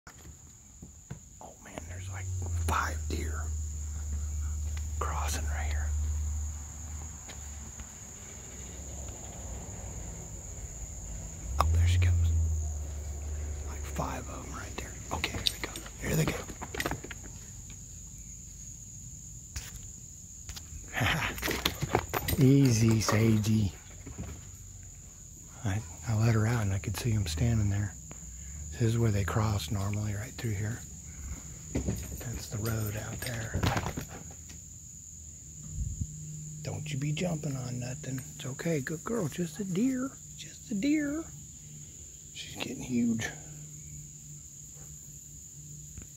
WHITETAIL deer running through back yard...FOREST.
WATCH as I first zoom. One is staring right at me,Then runs off. ANOTHER ZOOOOMS PAST.. ♡